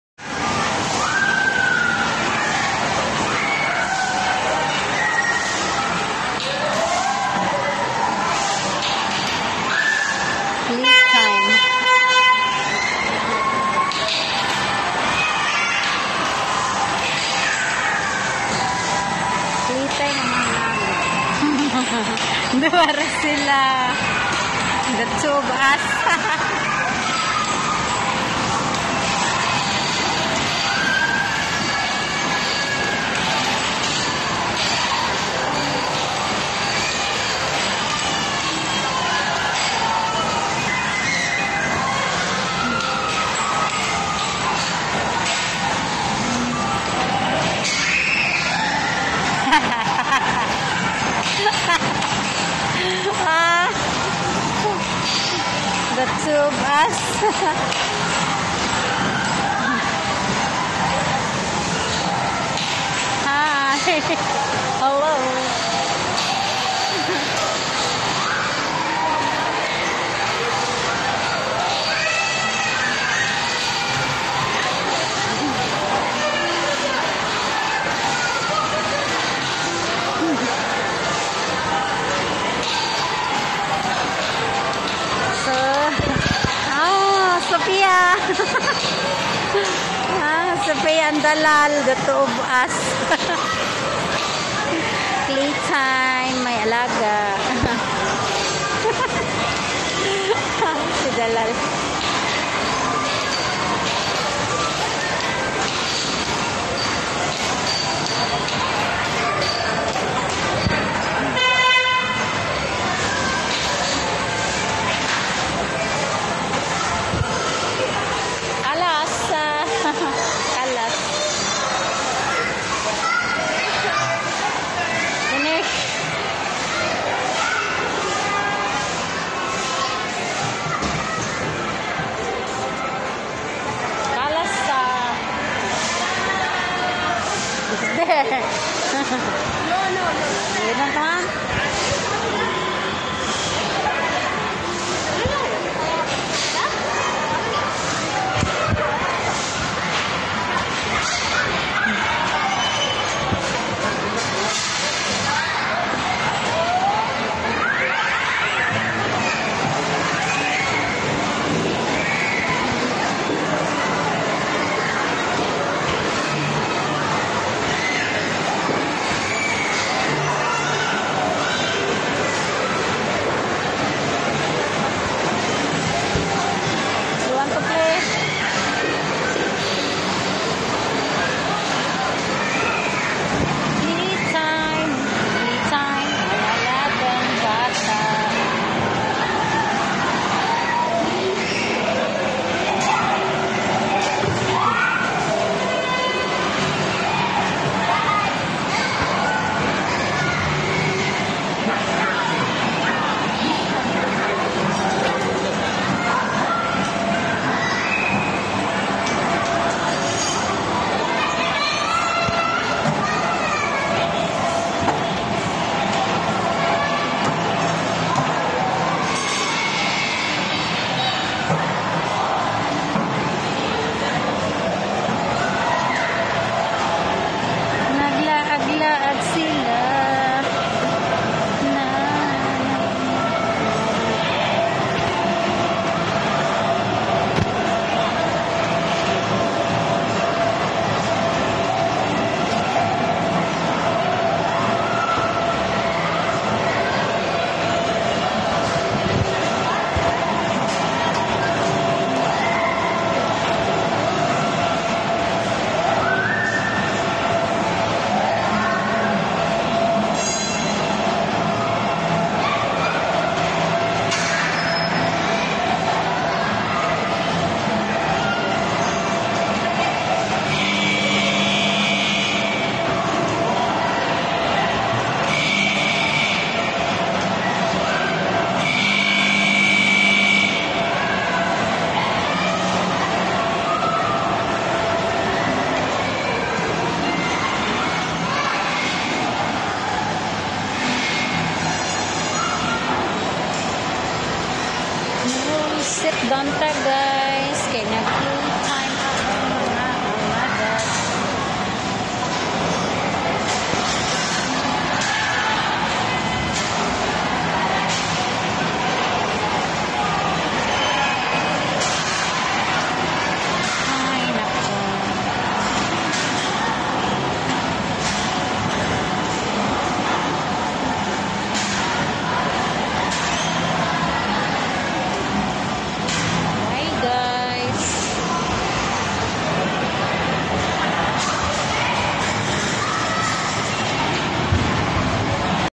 Ang aking Alaga at kanyang cousin play time
Gustong sumakay  ang aking
alaga na sila lang dalawa ng cousin niya nito